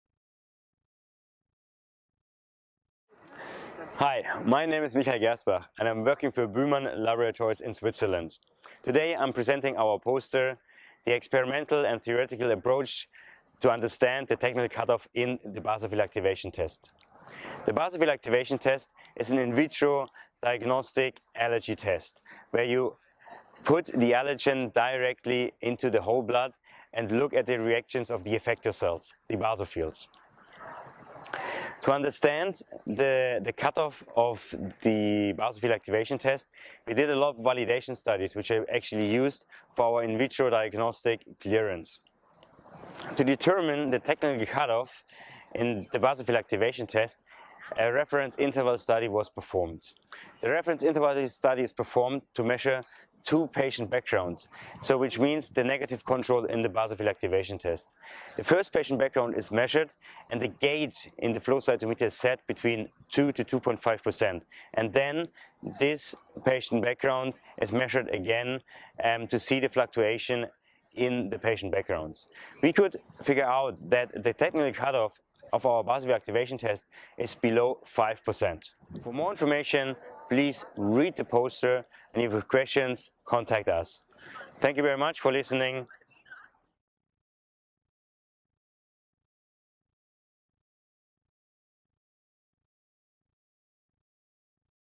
Experimental and Theoretical approaches to understand the Technical Cut-Off in BAT
With allergies being a global health threat, precise allergy diagnostics are critically important. This study demonstrates both experimental and theoretical approaches to understanding the technical cut-off in the Basophil Activation Test (BAT). This poster was presented at EAACI 2023.

Videos, photos and montage by Monika Anna Noga.